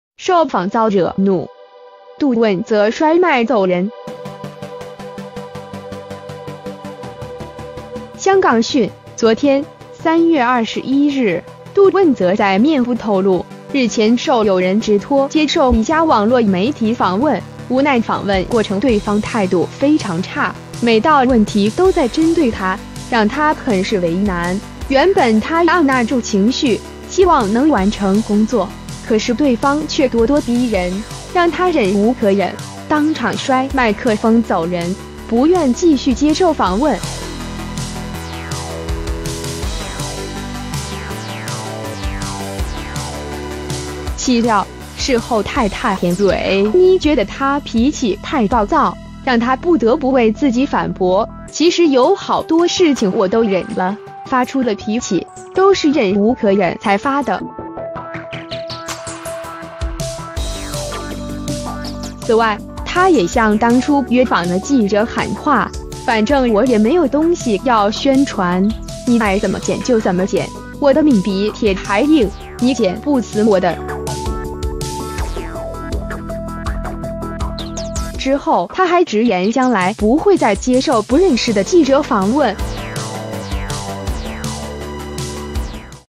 受访遭惹怒 杜汶泽摔麦走人
杜汶泽在面簿po文，写自己首次接受访问时半途除下麦克风走人。 （档案照）